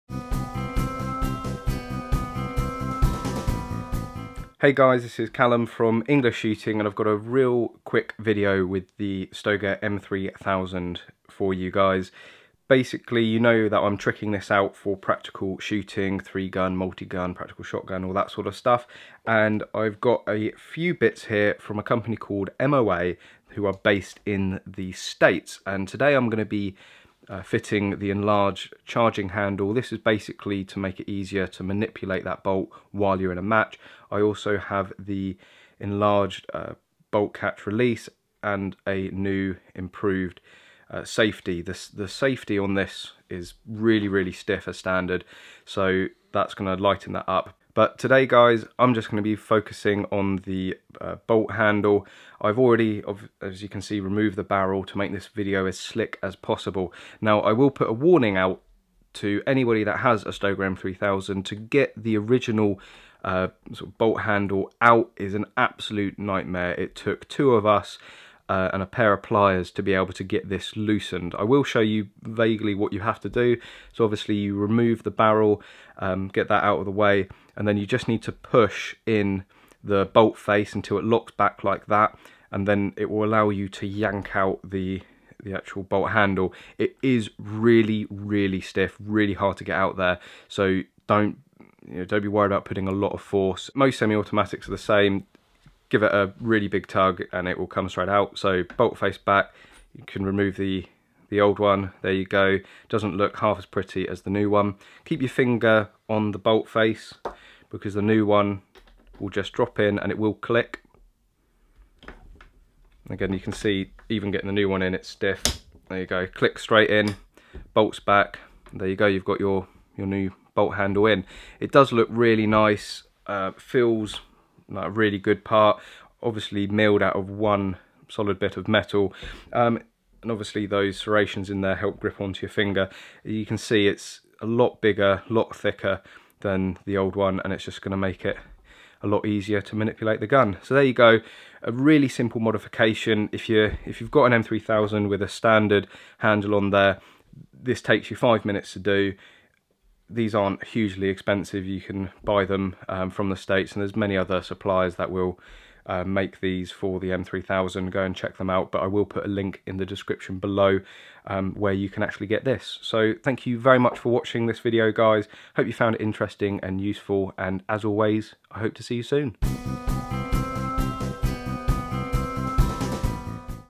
Stoeger M3000 MOA Bolt Handle Installation
The MOA Precision is one of the nicest bolt / charging handles for the Stoeger M3000 semi-auto shotgun. Perfect for adding better grip and more to grab during practical and multi gun / 3 gun shooting. Enjoy!

Equipment Used for Videos
Canon 700D Camera
Sandisk Extreme SD Card
RØDE VideoMic Pro Microphone
Velbon DF-51 Tripod
GoPro Hero Camera

Opinions are my own and do not necessarily reflect the opinions of any companies mentioned in the video.